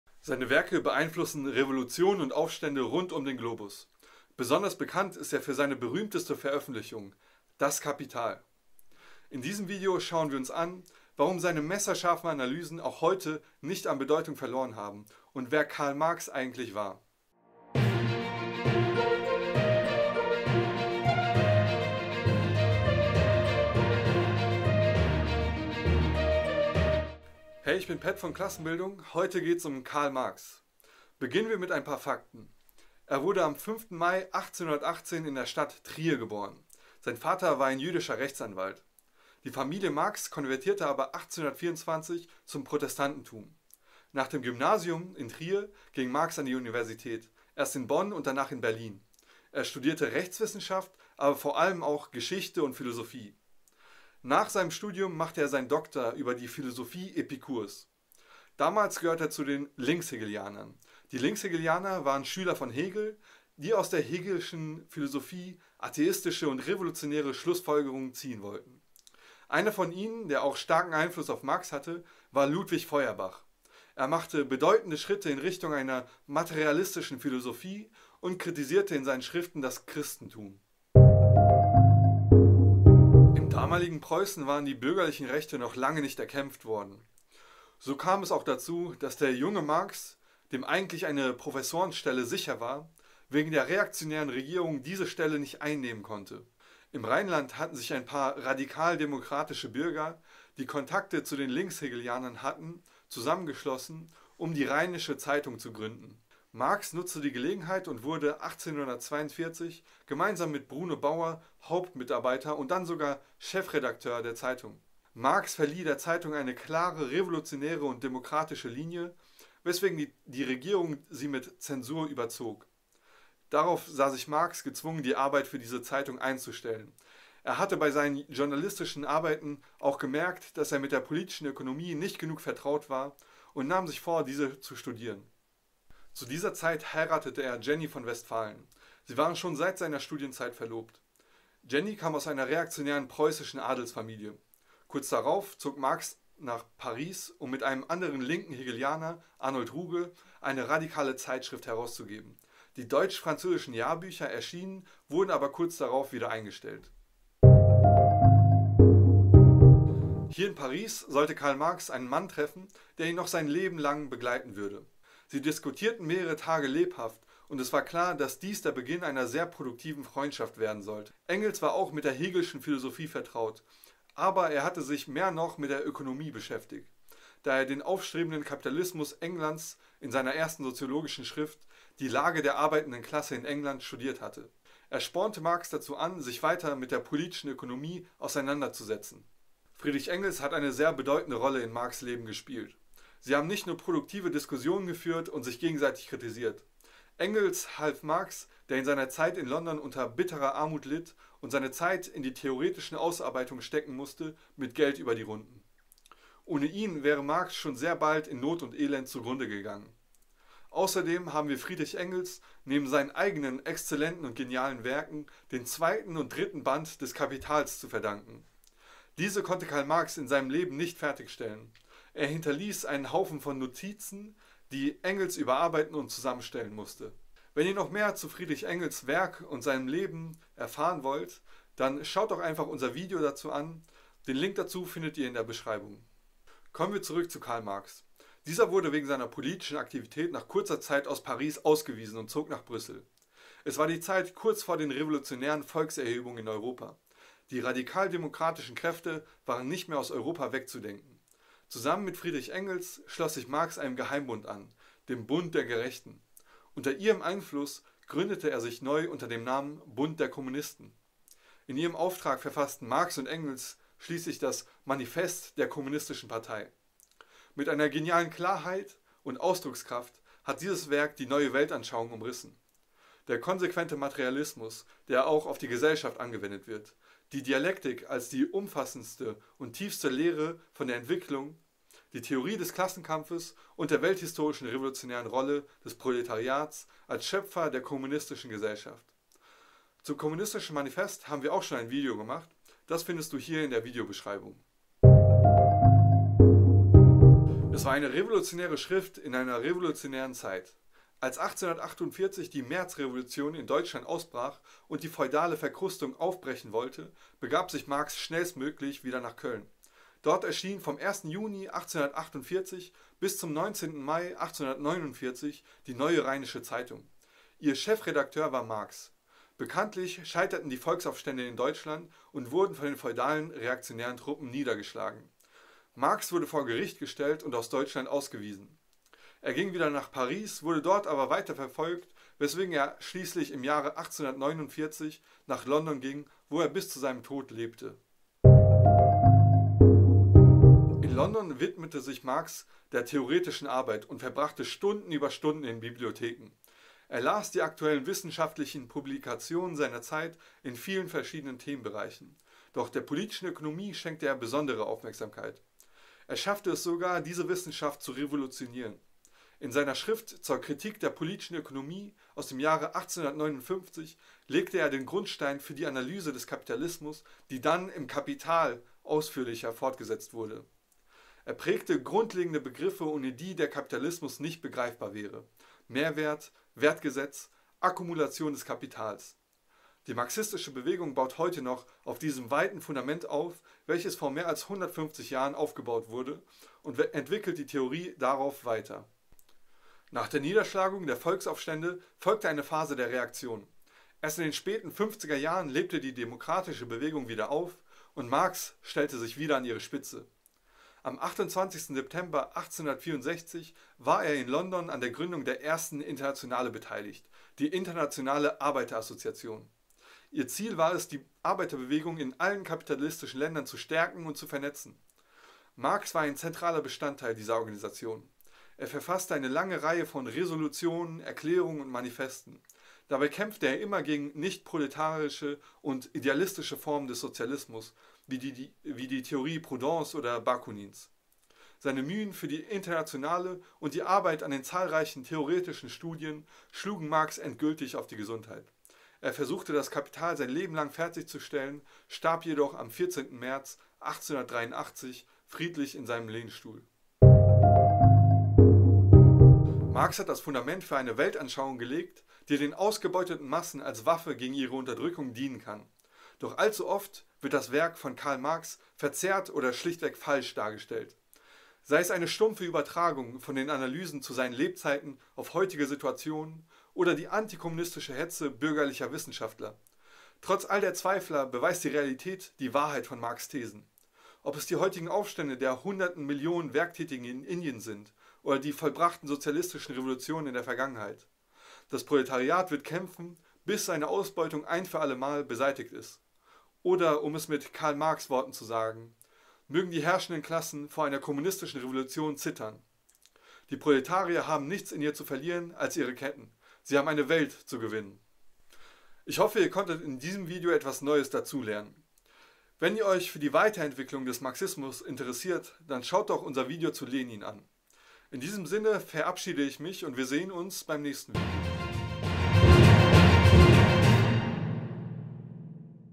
Wer war eigentlich Karl Marx?
Seine Werke beeinflussen Revolutionen und Aufstände rund um den Globus. Besonders bekannt ist er für seine berühmteste Veröffentlichung: Das Kapital. In diesem Video schauen wir uns an, warum seine messerscharfen Analysen auch heute nicht an Bedeutung verloren haben und wer KarlMarx eigentlich war.

0:00 Intro
0:27 Fakten über Karl Marx
1:21 Rheinische Zeitung
3:38 Der beginn des gemeinsamen Kampfes und einer großen Freundschaft
4:55. Aufstände in Europa
5:39 Im Londonder Exil
7:28 Marxismus Heute